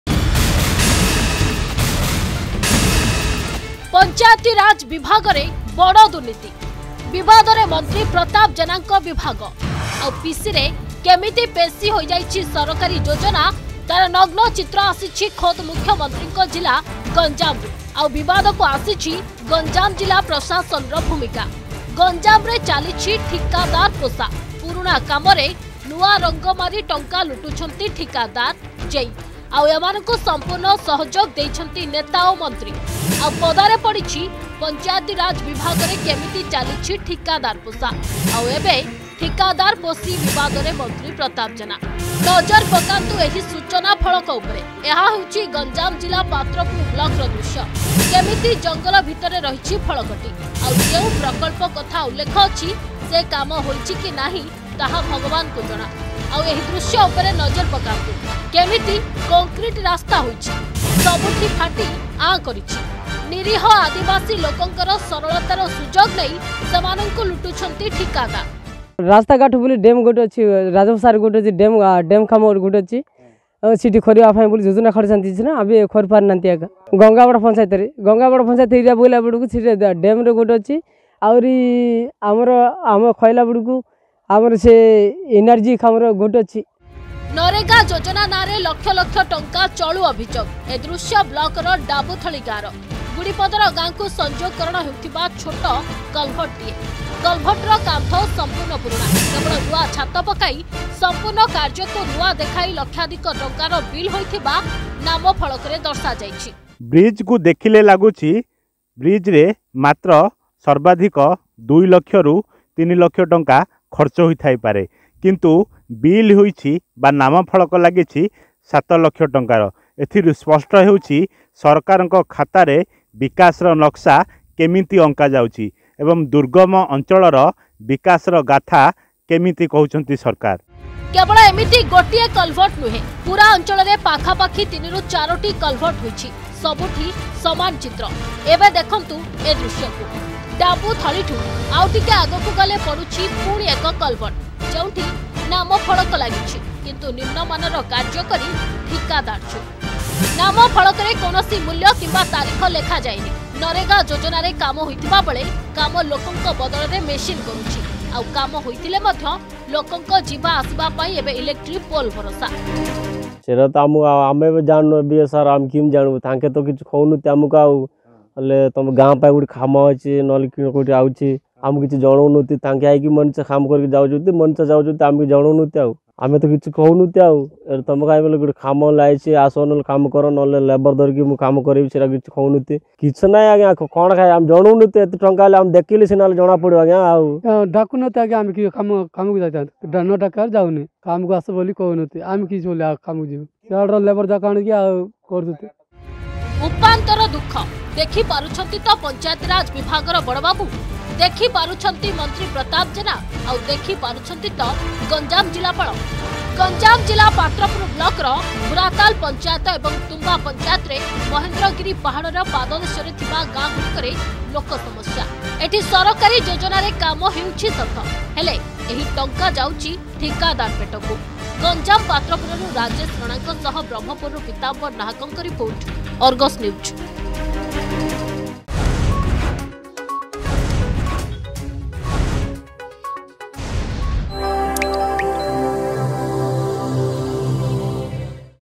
MGNREGA Scam in Ganjam, Pratap Jena's Panchayati Raj Department Embroiled in Controversy
MGNREGA Scam in Ganjam, Pratap Jena's Panchayati Raj Department Embroiled in Controversy. Contractors and Government Engineers Fill Their Pockets at the cost of Real Beneficiaries Suffering.